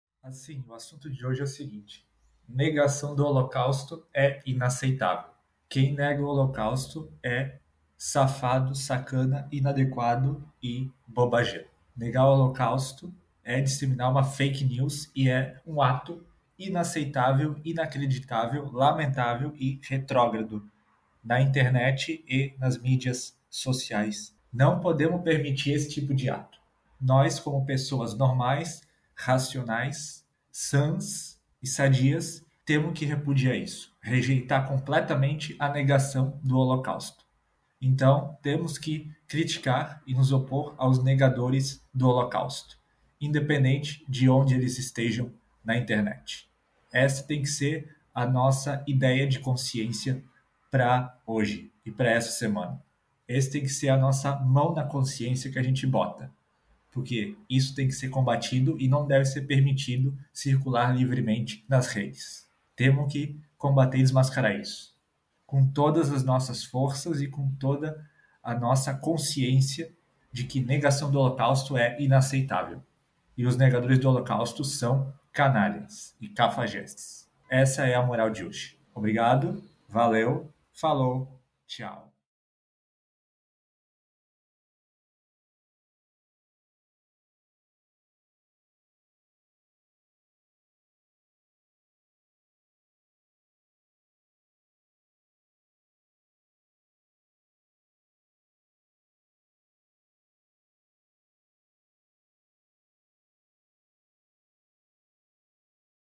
Vlog 252 - Os Negadores BR Do Holocausto.
Nesse vídeo, eu falo sobre a negação do Holocausto na internet.

Wikimedia Commons
Pixabay
3rd Division SS Totenkopf (Logotipo)